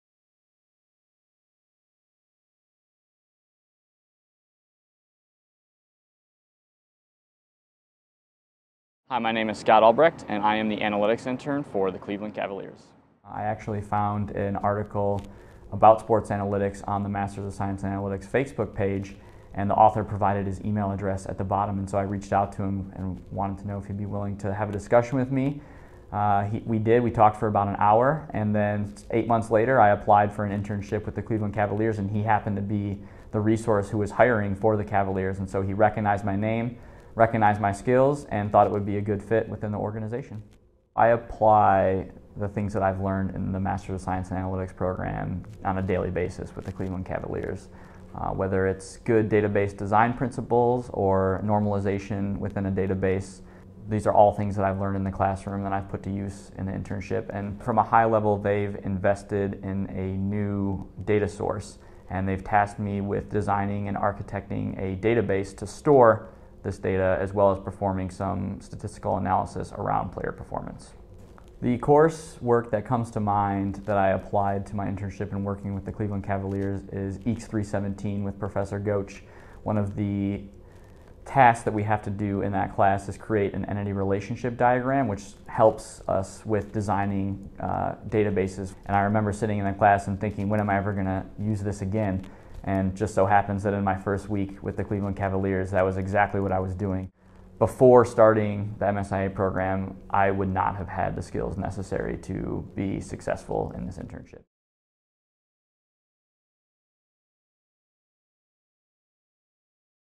Northwestern MSiA: Scott Albrecht '13, Sports Analytics Intern
- Northwestern University's Master of Science in Analytics (MSiA) Program requires a 10-week student internship. Students reaching the internship phase have completed three-quarters of their coursework and are highly motivated to apply their knowledge and skills to a professional setting.

Scott Albrect, MSiA '13, fulfilled his internship requirement with the Cleveland Cavaliers as a Sports Analytics Intern.